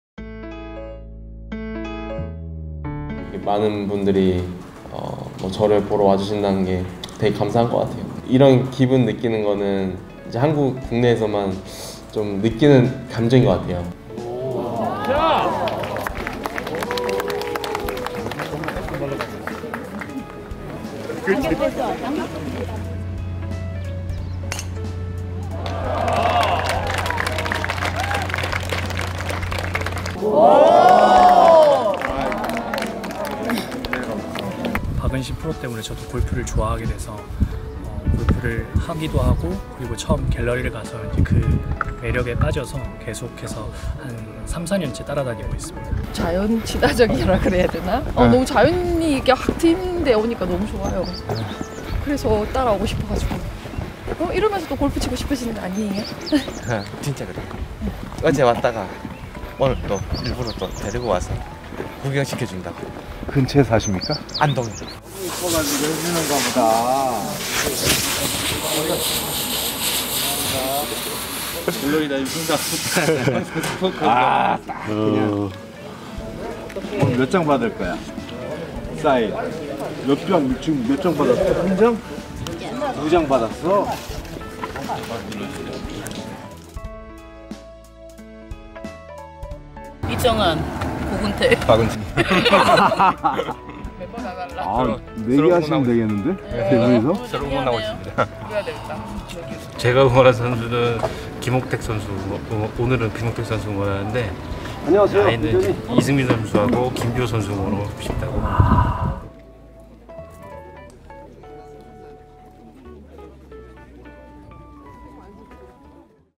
[인터뷰다큐]KPGA대회를 만드는 사람들_EP1 갤러리편
KPGA대회를 만들고 완성시키는 수많은 사람들

그 첫번째 주인공 '갤러리'를 만나러 갑니다.